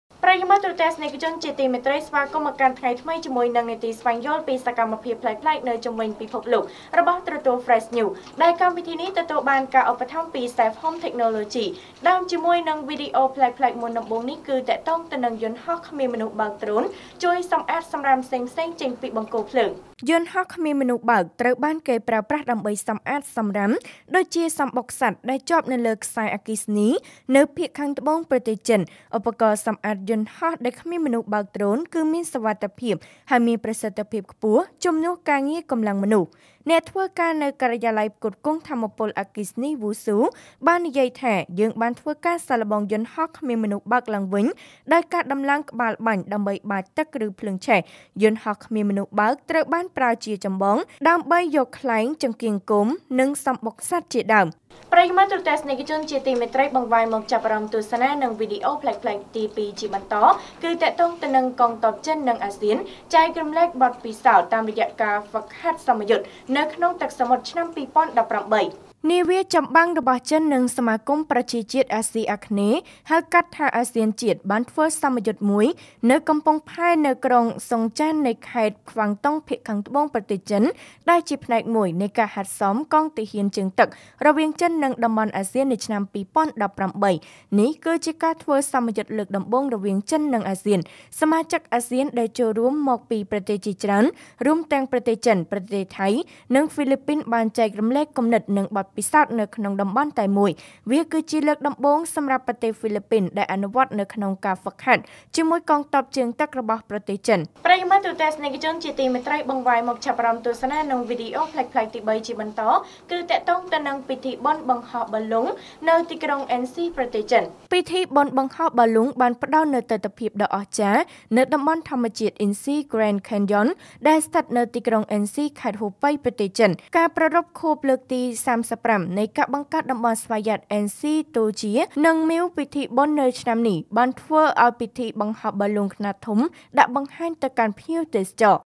ទស្សនា​វីដេអូ​​ប្លែកៗ​ជុំវិញ​ពិភពលោក
១៖ យន្តហោះគ្មានមនុស្សបើក Drone ជួយសម្អាត សម្រាម ផ្សេងៗ ចេញពីបង្គោលភ្លើង
២៖ កងទ័ពចិន និងអាស៊ាន ចែករំលែកបទពិសោធន៍ តាមរយៈការហ្វឹកហាត់សមយុទ្ធ នៅក្នុងទឹកសមុទ្រឆ្នាំ 2018
៣៖ ពិធីបុណ្យបង្ហោះប៉ាឡុង ផ្តល់នូវទិដ្ឋភាពដ៏អស្ចារ្យនៅទីក្រុង Ensh ប្រទេសចិន